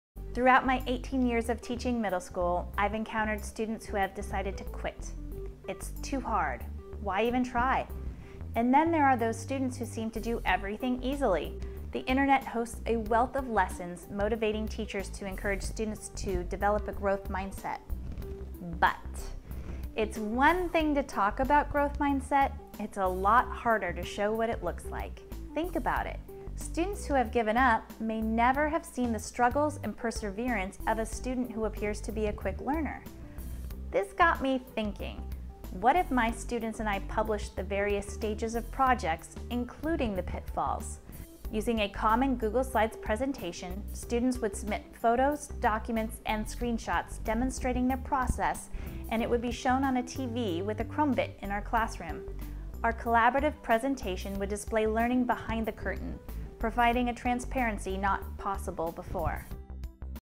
Google Certified Innovator Application Video
I've seen several students "give up" on school because they believe they are not smart enough. My belief is that they've only seen the final products of "smart" students and not the hard work and struggles.To support the shift to a growth mindset, I propose that we publicize the behind the scenes work. Learning is messy and students need to see the process of learning and not just the end result.